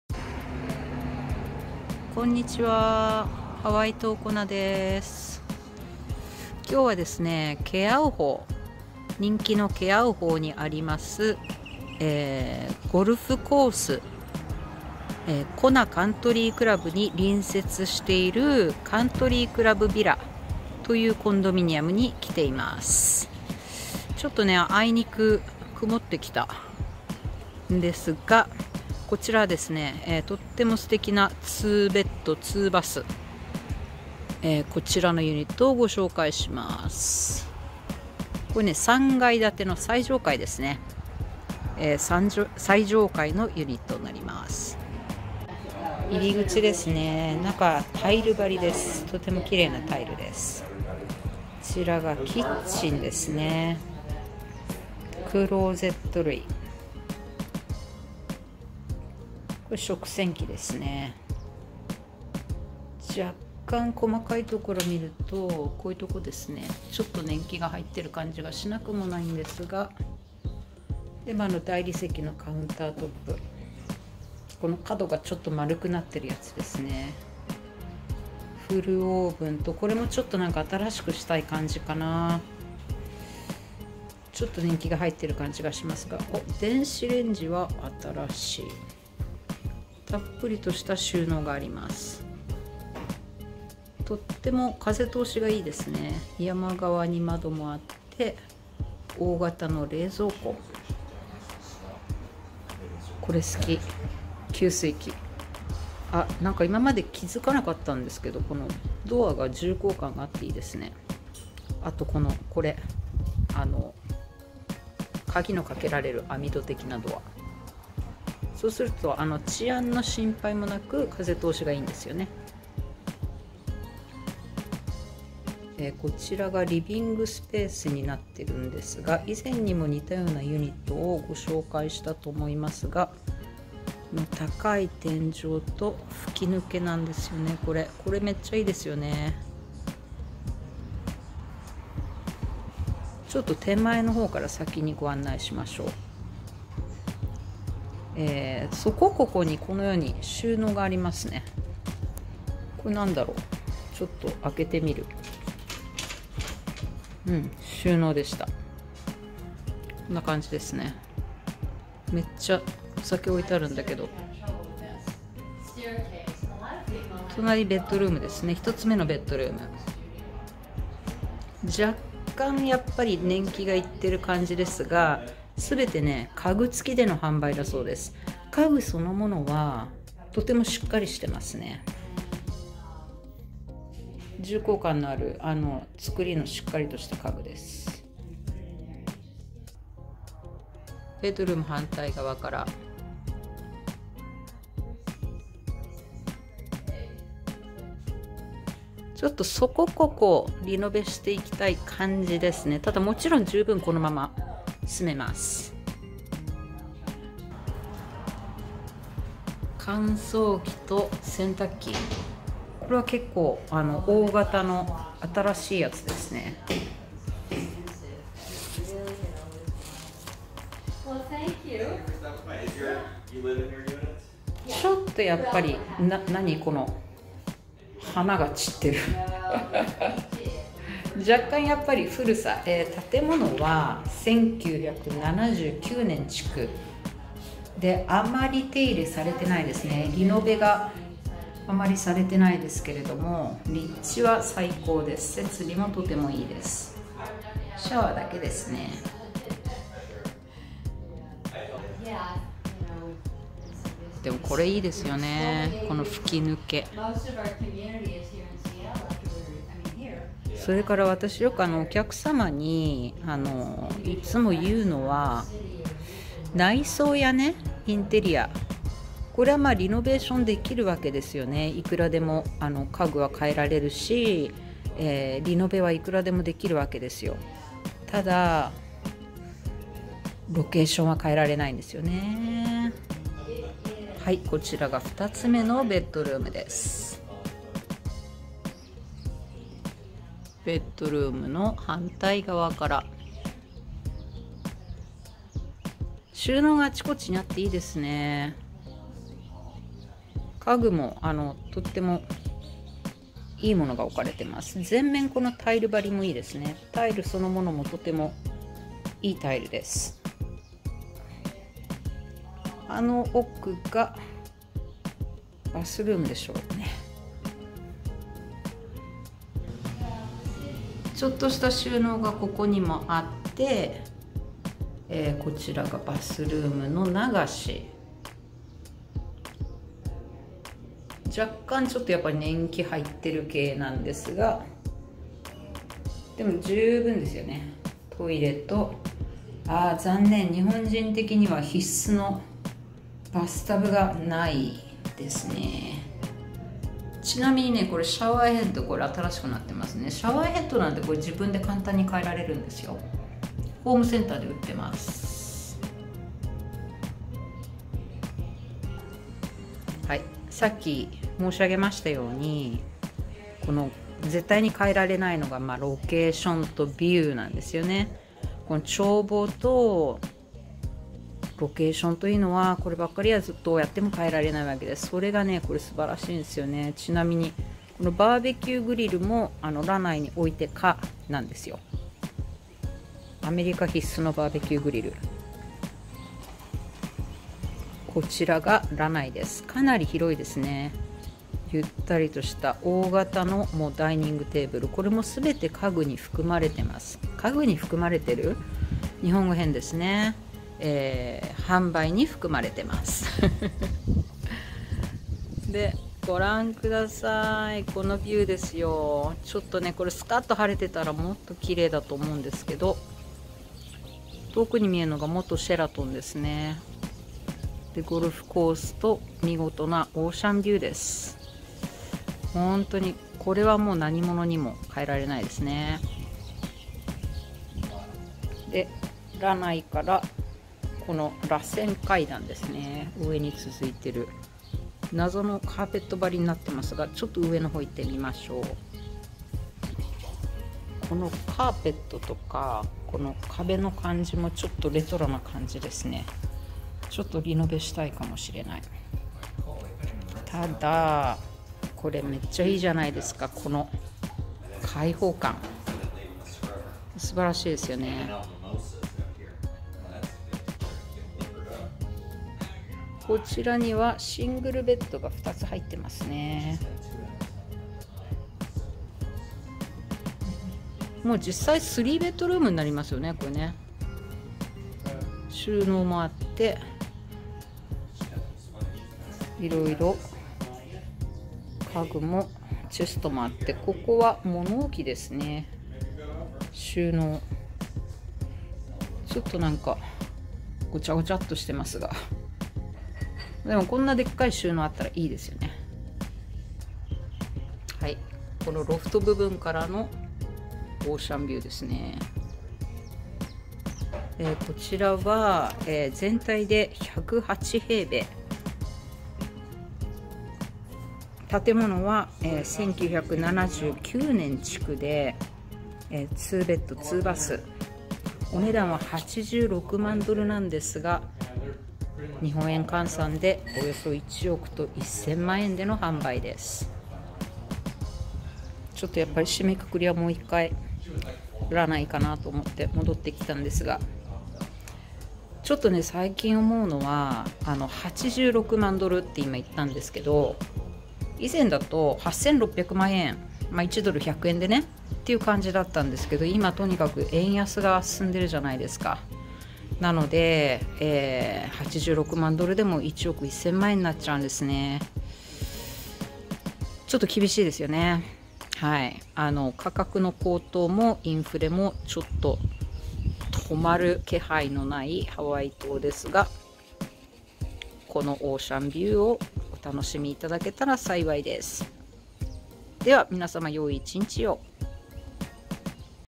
ハワイ島コナ2ベッドコンド約1.1億円 SOLD・売却済
短期バケレンは7日以上から可能。家具付き、ロフト付き、ゴルフ場隣接のコンドです。